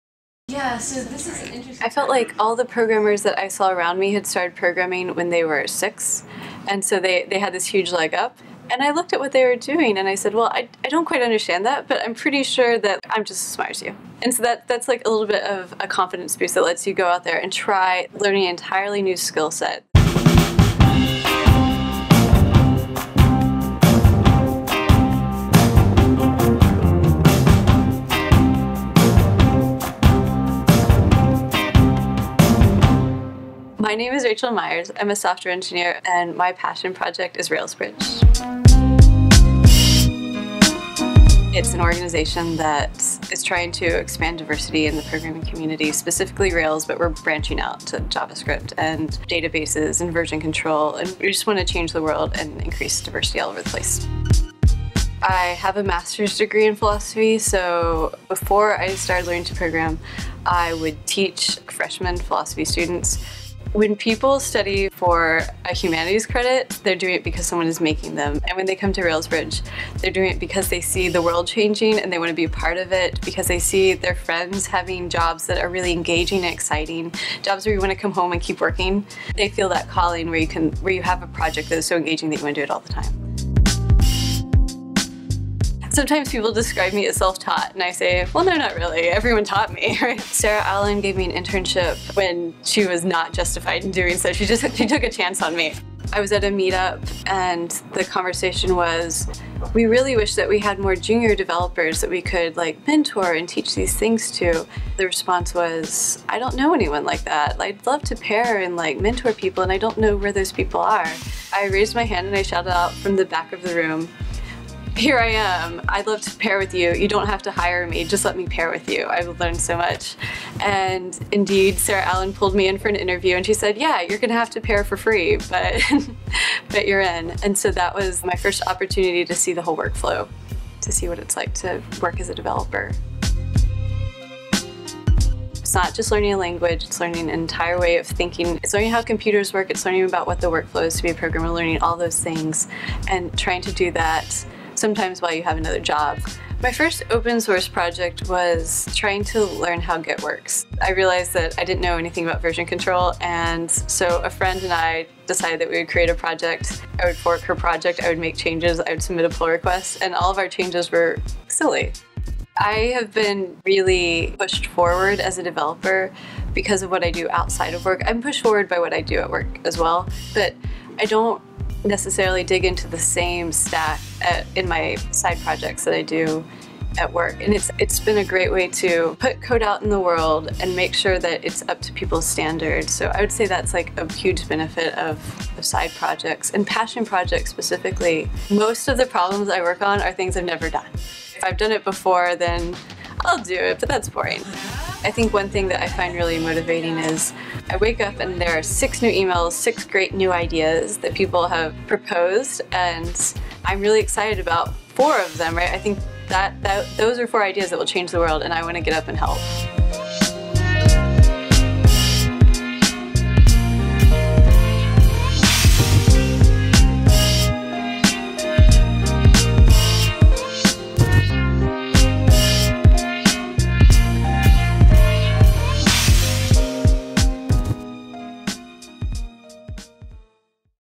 Passion Projects (Docs) • Rachel Myers (RailsBridge)
First installment of our Passion Projects short documentary series, featuring Rachel Myers. Rachel is a Rubyist at GitHub and open source enthusiast who volunteers with the non-profit organization RailsBridge to teach (more) women to code.

Thanks!